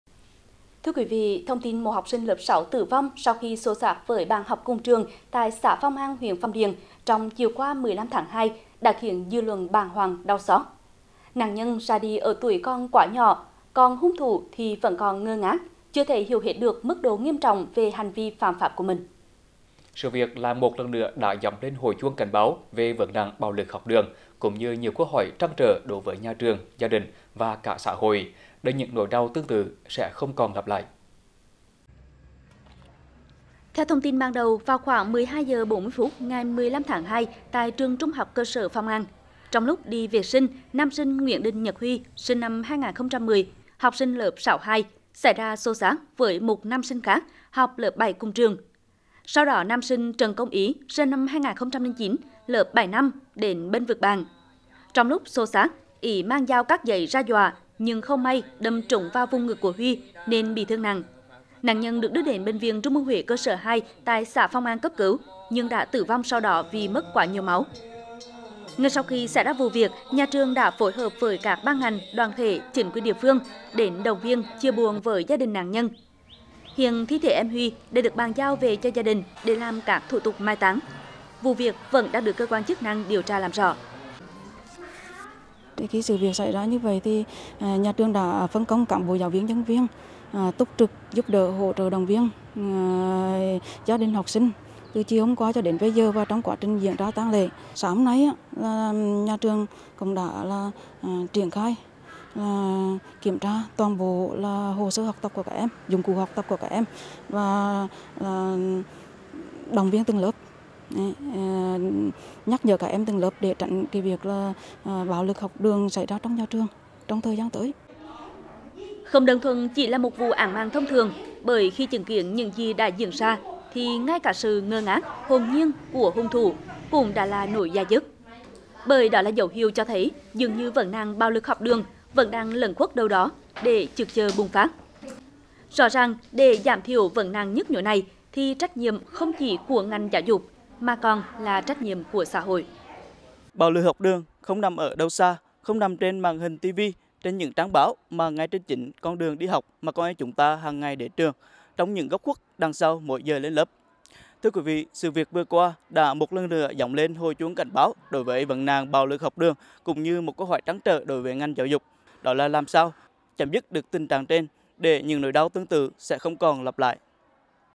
Xô xát trong trường học dẫn đến cái chết của một học sinh lớp 6
Thông tin một học sinh lớp 6 tử vong sau khi xô xát với bạn học cùng trường tại xã Phong An, huyện Phong Điền trong chiều qua 15/2 đã khiến dư luận bàng hoàng, đau xót. Nạn nhân thì ra đi ở tuổi còn quá nhỏ còn hung thủ thì vẫn còn ngơ ngác, chưa thể hiểu hết được mức độ nghiêm trọng về hành vi phạm pháp của mình.
Sự việc  lại một lần nữa đã gióng lên hồi chuông cảnh báo về vấn nạn bạo lực học đường cũng như nhiều câu hỏi trăn trở đối với nhà trường, gia đình và cả xã hội để những nỗi đau tương tự sẽ không còn lặp lại